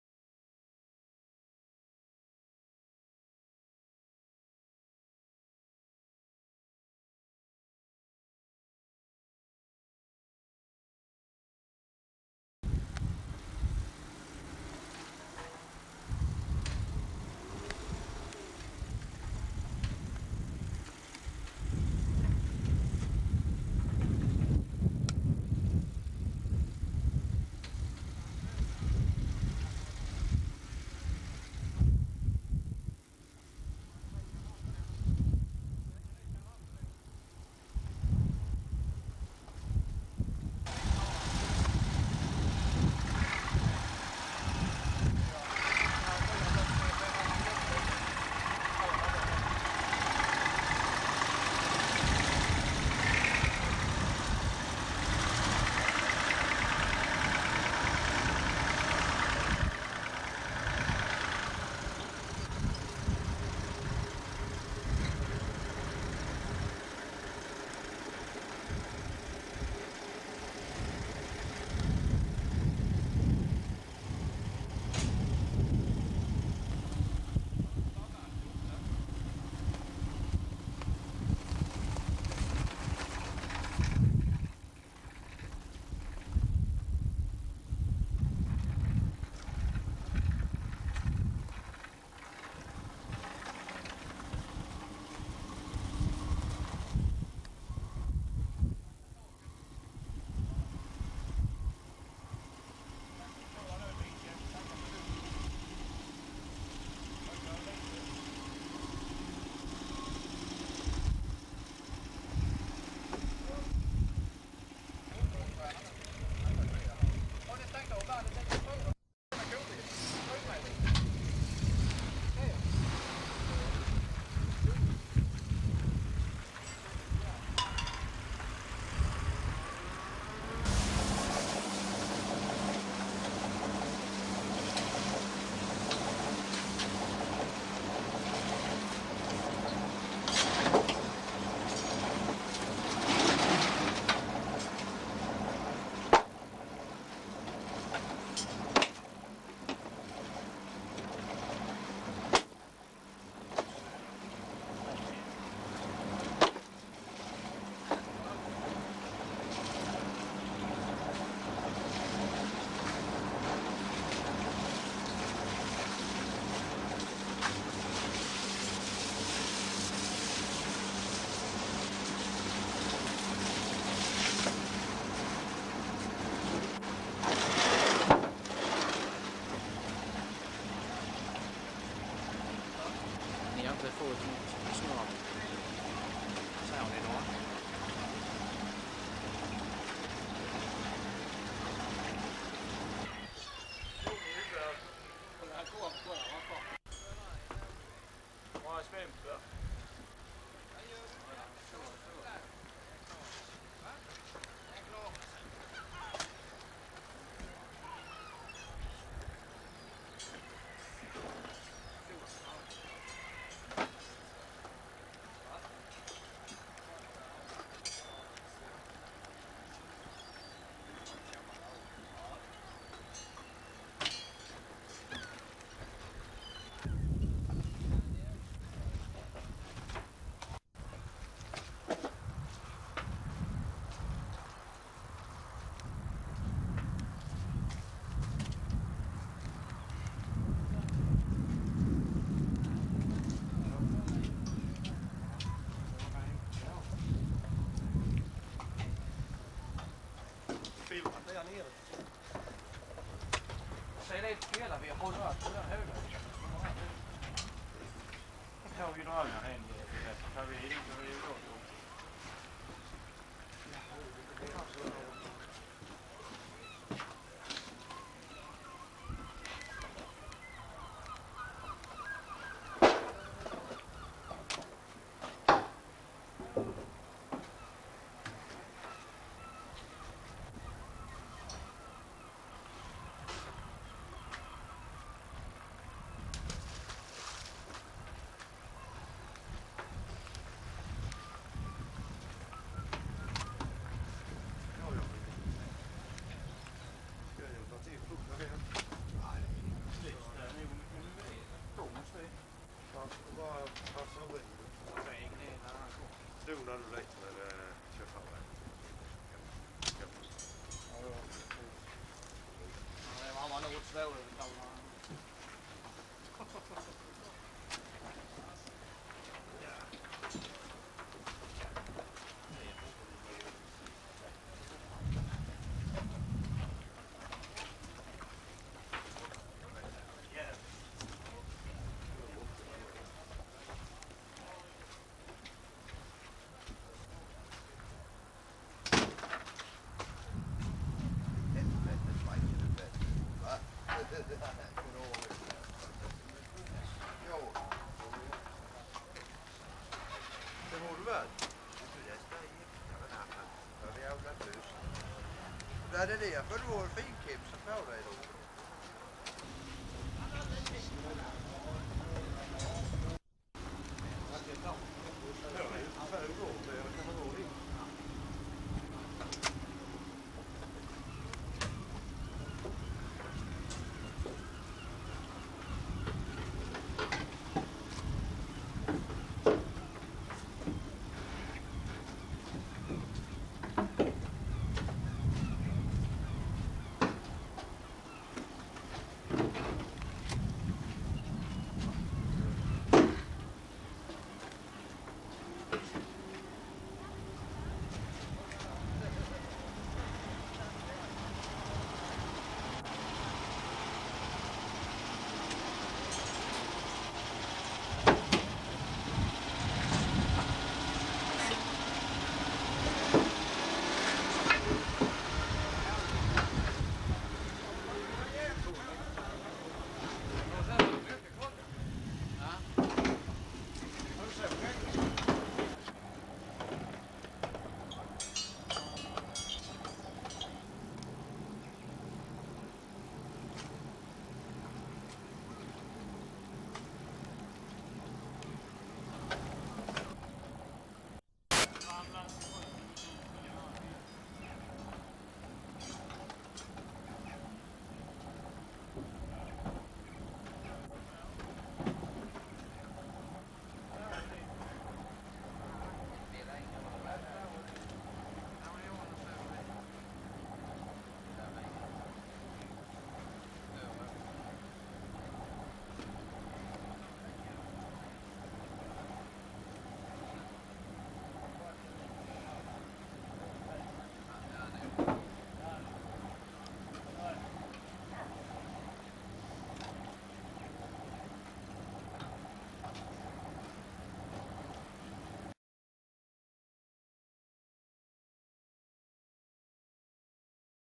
Fiskare vid Ekenäs brygga på Senoren
Fiskare och andra glada gubbar nere vid Ekenäs brygga, då det fortfarande var liv och rörelse nere vid bryggan. Film av Bengt Emanuel Svensson.